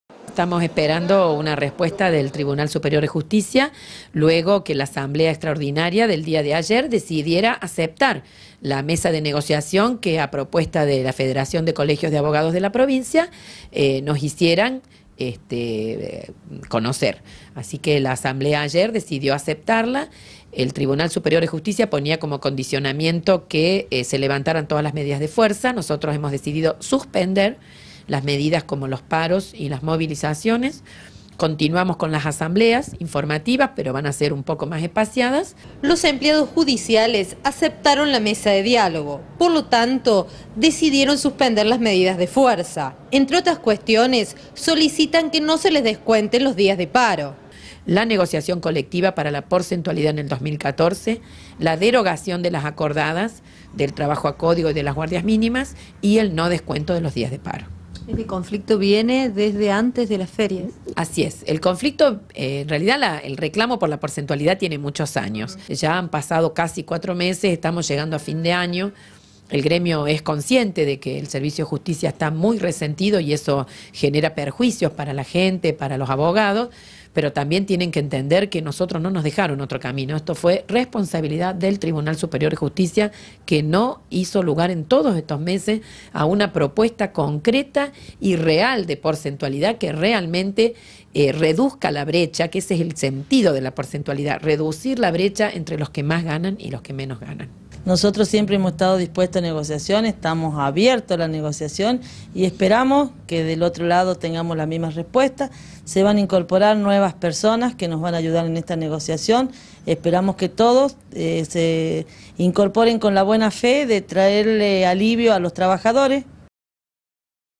Judiciales aceptaron participar de la mesa de negociación
Los empleados del Poder Judicial levantaron todas las medidas de protesta y se suman a la mesa de negociación con el TSJ.